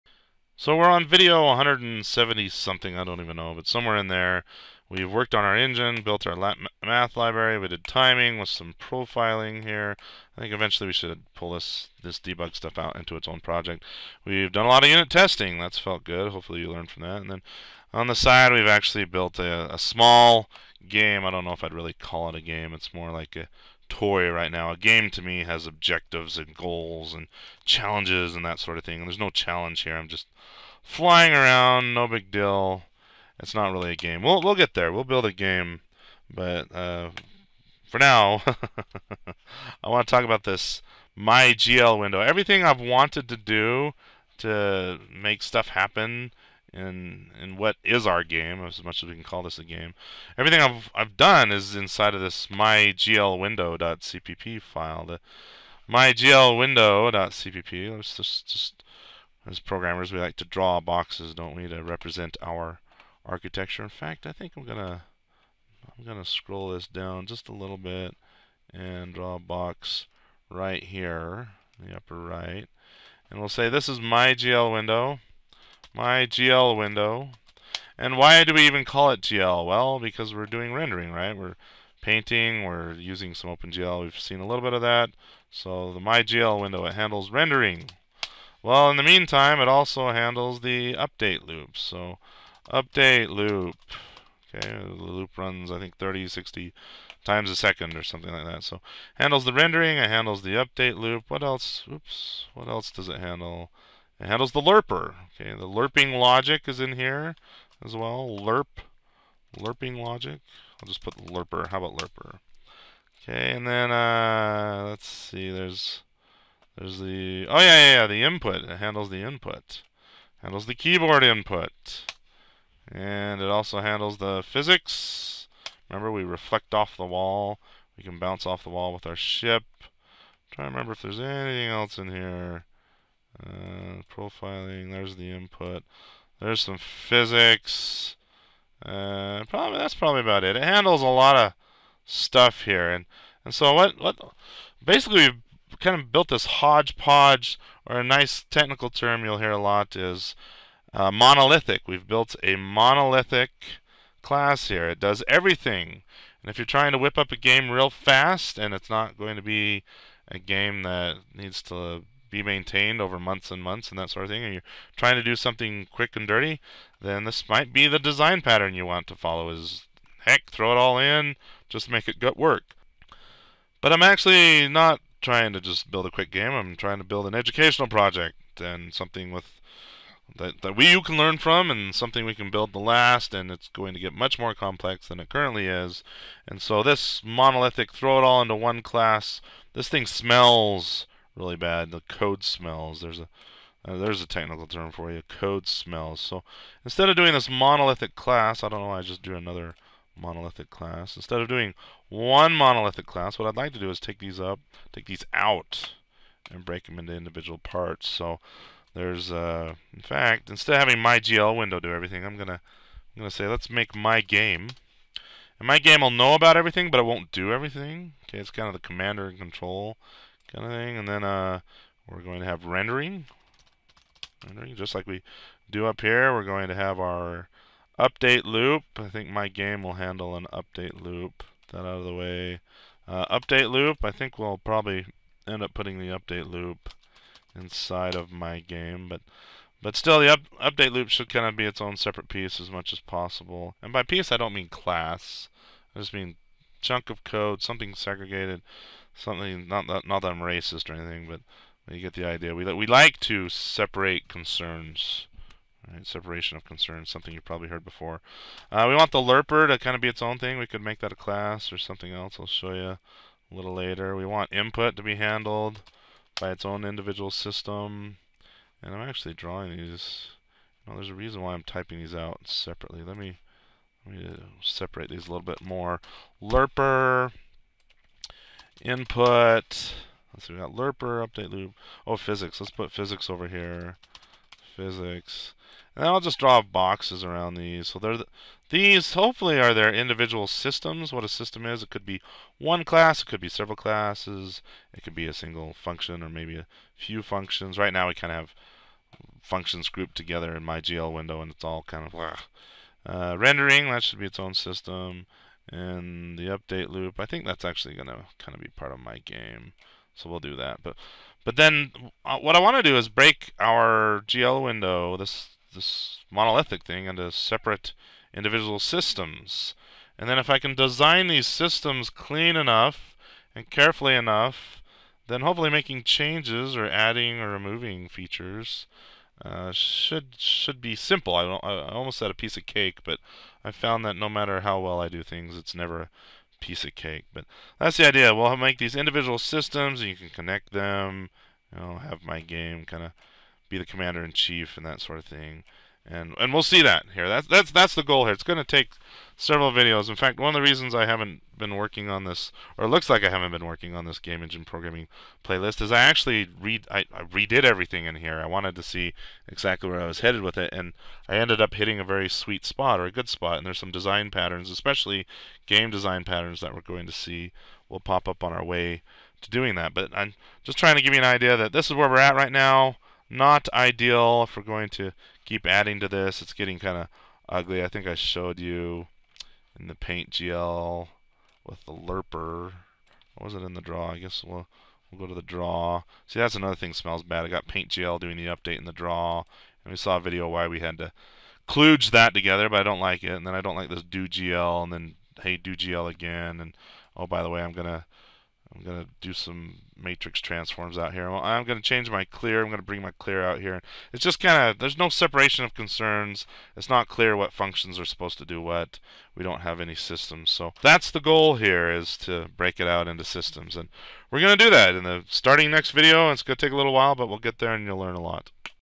Code Smells-Game Engine Systems Redesign-Overhaul-Refactor Intro
Our code smells bad, and we need to refactor, redesign, and clean it up. We also need to break our monolithic class into separate systems.